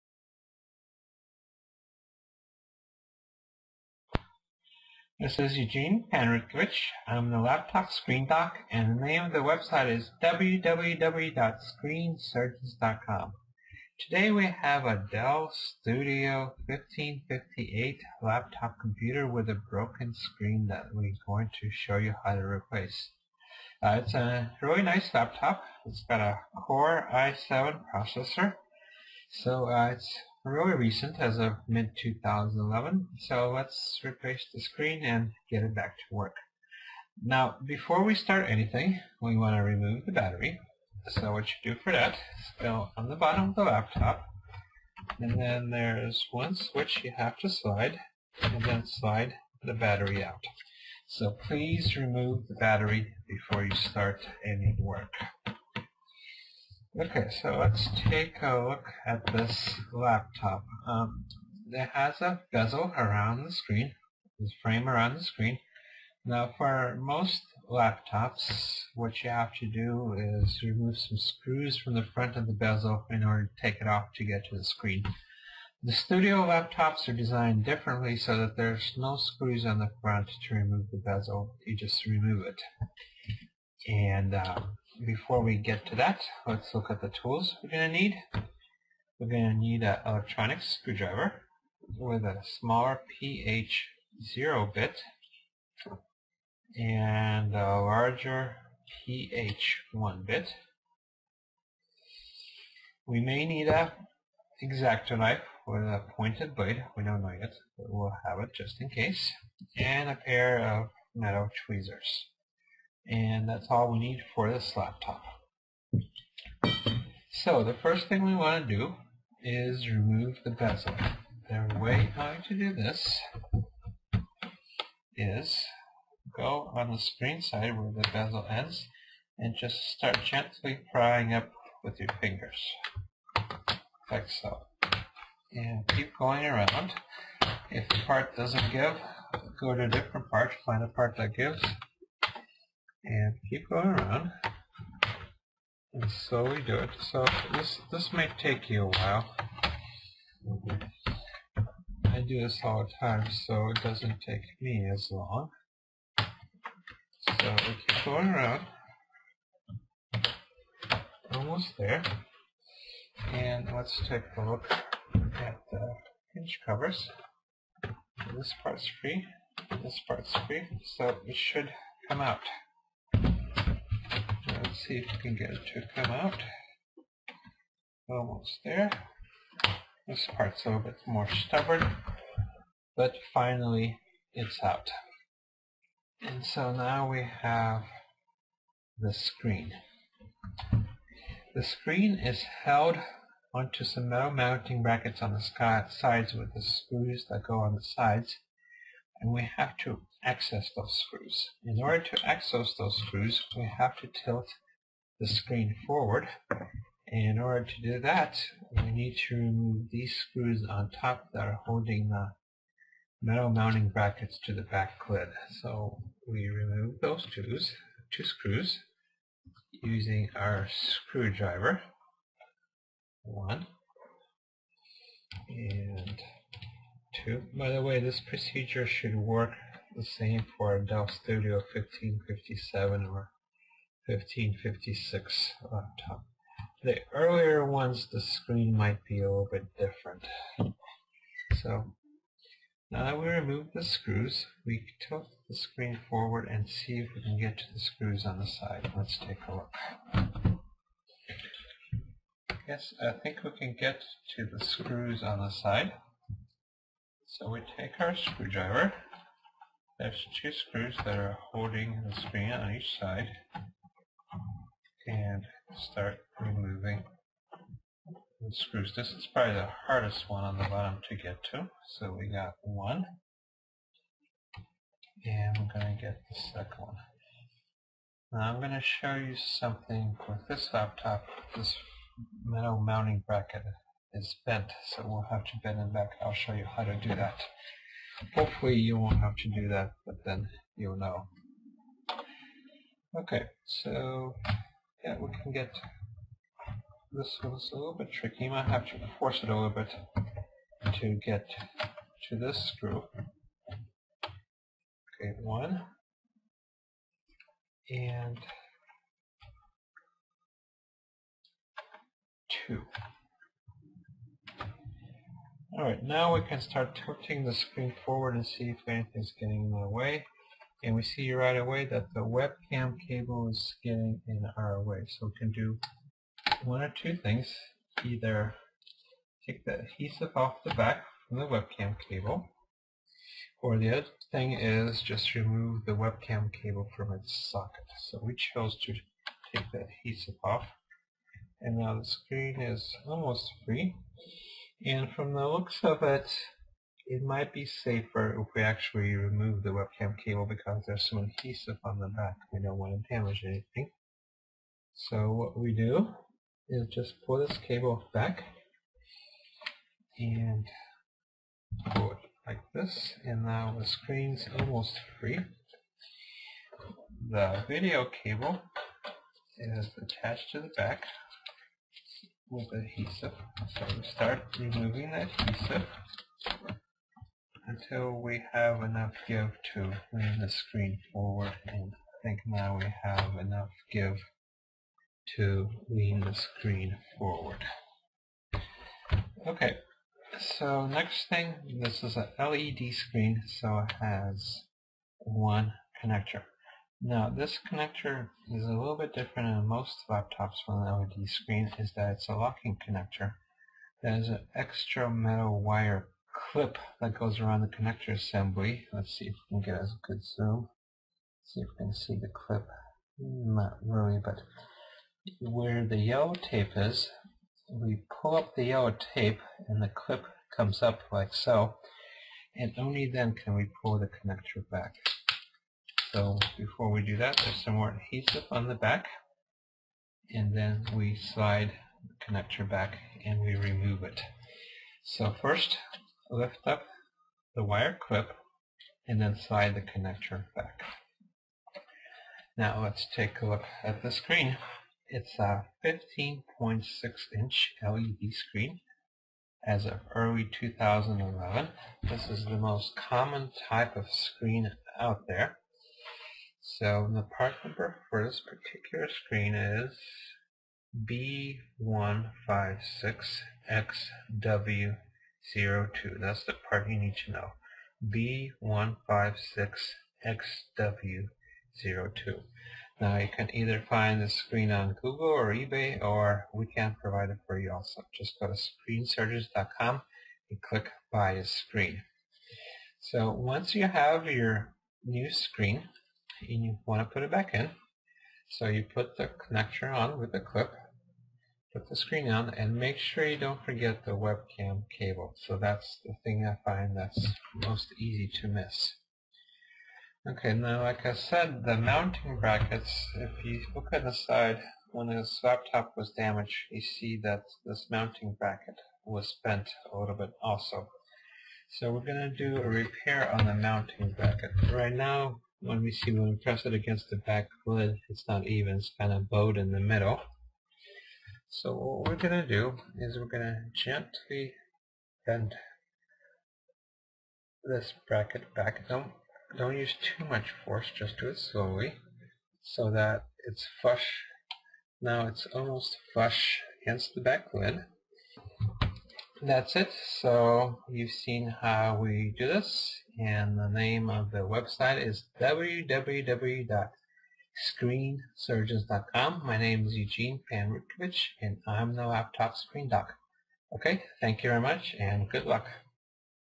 Dell Studio 1558 Laptop Screen Replacement Procedure
Step by step instructions to repair a damaged laptop LCD screen on a Dell Studio 1558 laptop computer.